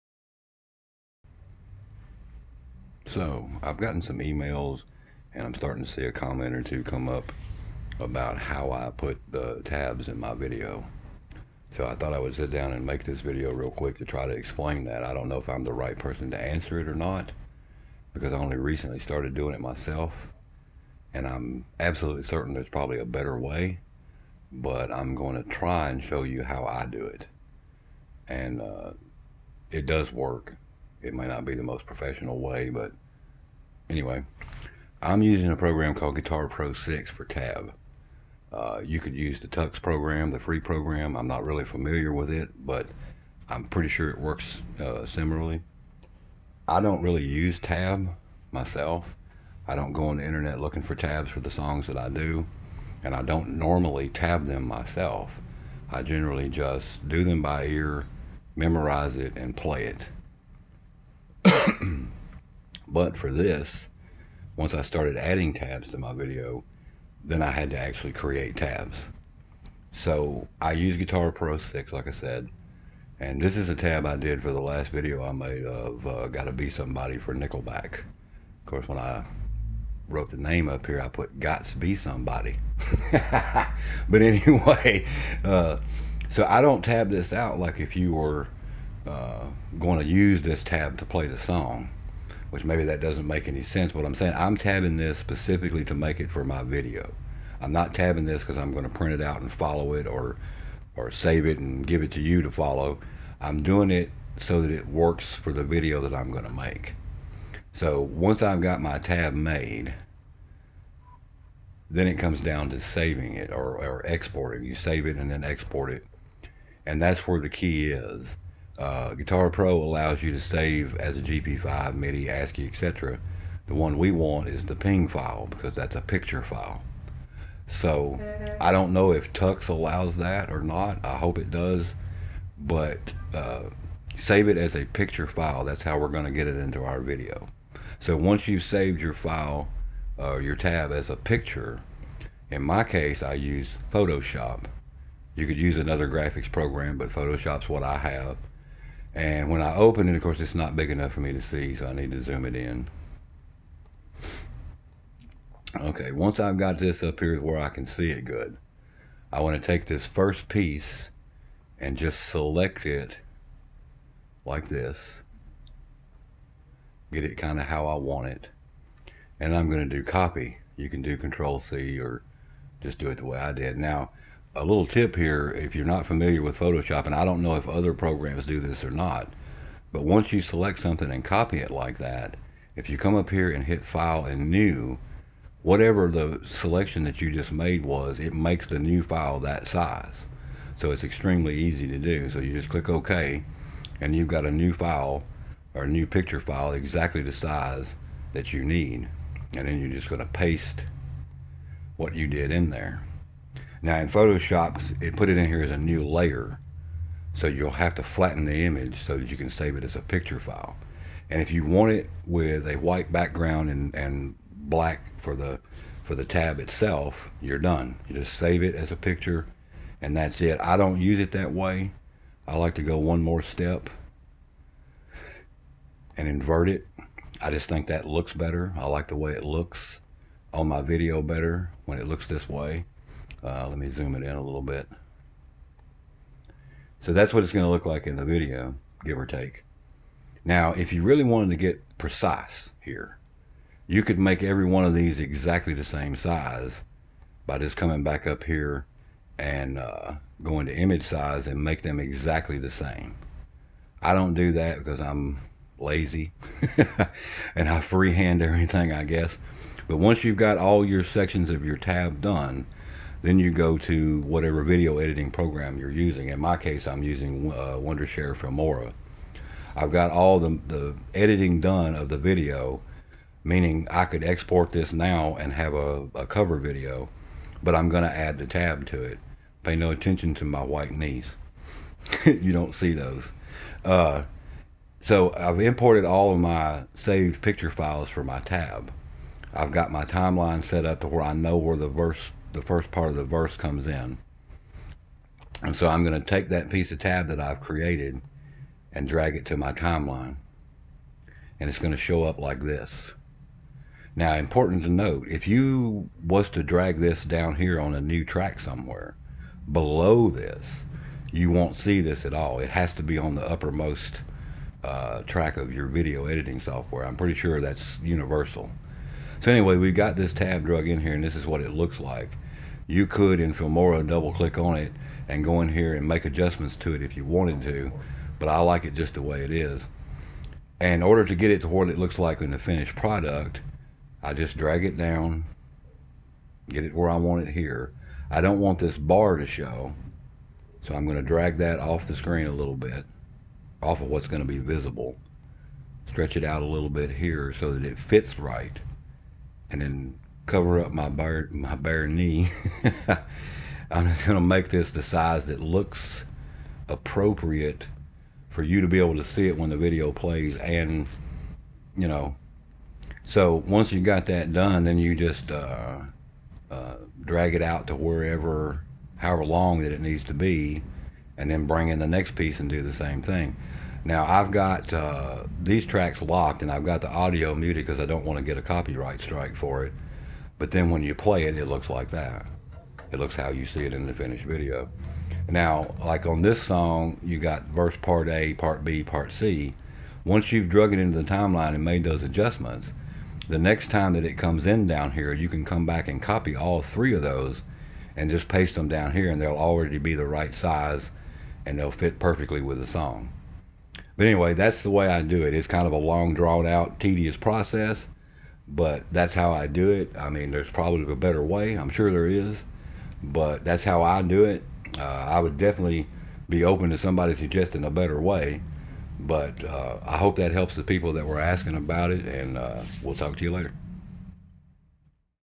How I Add TABS to my YOUTUBE videos.
A quick tutorial on how I add the tabs to my youtube videos.